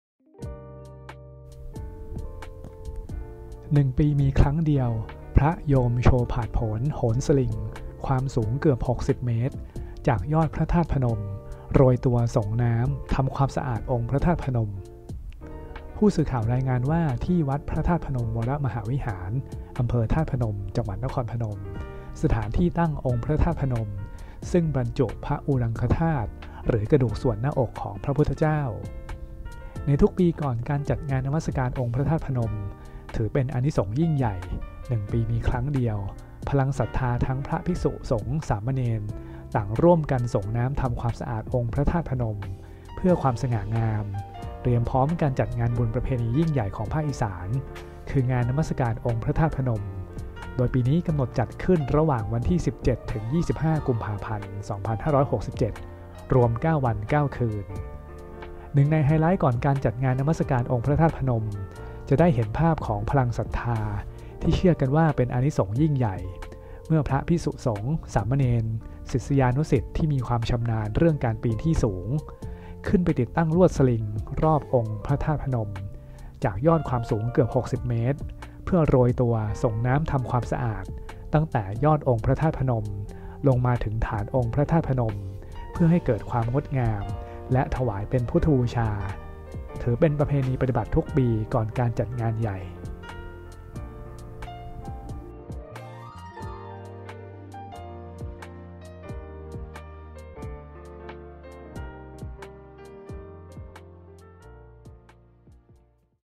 พระ โยม โชว์ผาดโผนโหนสลิง สรงน้ำทำความสะอาดองค์พระธาตุพนม
ผู้สื่อข่าวรายงานว่า ที่ “วัดพระธาตุพนมวรมหาวิหาร” อ.ธาตุพนม จ.นครพนม สถานที่ตั้งองค์พระธาตุพนมซึ่งบรรจุพระอุรังคธาตุ หรือกระดูกส่วนหน้าอกของพระพุทธเจ้า

ในทุกปี ก่อนจัดงานนมัสการองค์พระธาตุพนม ถือเป็นอานิสงส์ยิ่งใหญ่ หนึ่งปีมีครั้งเดียว พลังศรัทธา ทั้งพระภิกษุสงฆ์ สามเณร ต่างร่วมกันสรงน้ำทำความสะอาดองค์พระธาตุพนม เพื่อความสง่างามเตรียมพร้อม การจัดงานบุญประเพณียิ่งใหญ่ของภาคอีสาน คือ “งานนมัสการองค์พระธาตุพนม” โดยปีนี้กำหนดจัดขึ้น ระหว่างวันที่ 17 – 25 กุมภาพันธ์ 2567 รวม 9 วัน 9 คืน

หนึ่งในไฮไลต์ก่อนการจัดงานนมัสการองค์พระธาตุพนม จะได้เห็นภาพของพลังศรัทธา ที่เชื่อกันว่าเป็นอานิสงส์ยิ่งใหญ่ เมื่อพระภิกษูสงฆ์ สามเณร ศิษยานุศิษย์ ที่มีความชำนาญ เรื่องการปีนที่สูง ขึ้นไปติดตั้งลวดสลิง รอบองค์พระธาตุพนม จากยอดความสูงเกือบ 60 เมตร เพื่อโรยตัว สรงน้ำทำความสะอาด ตั้งแต่ยอดองค์พระธาตุพนม ลงมาถึงฐานองค์พระธาตุพนม เพื่อให้เกิดความงดงาม  และถวายเป็นพุทธบูชา ถือเป็นประเพณีปฏิบัติทุกปี ก่อนการจัดงานใหญ่